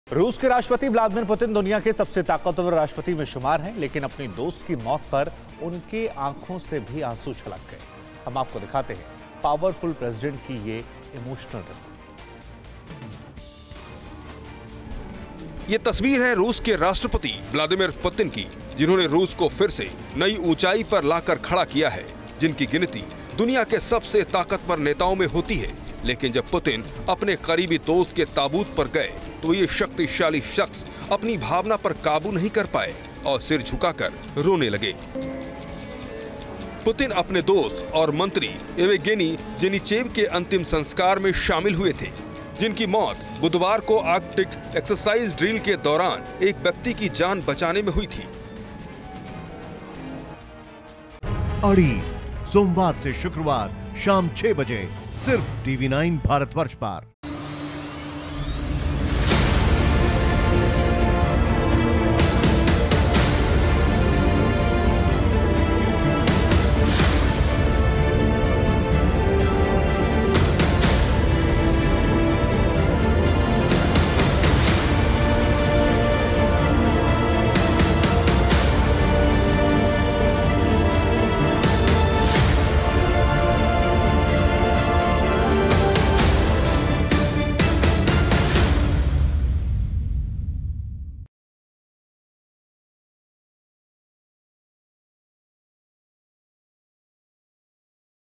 रो पड़ा पावरफुल प्रेसिडेंट, पुतिन को दोस्ता का जाना रुला गया । Vladimir Putin Cried
रूस के राष्ट्रपति ब्लादिमीर पुतिन दुनिया के सबसे ताकतवर राष्ट्रपति में शुमार है. लेकिन अपनी दोस्त की मौत पर उनके आंसू छलक गए. देखिए पावरपुल प्रेसिडेंट की ये इमोशनल रिपोर्ट .

Credit: International । ShubhankarMishra ।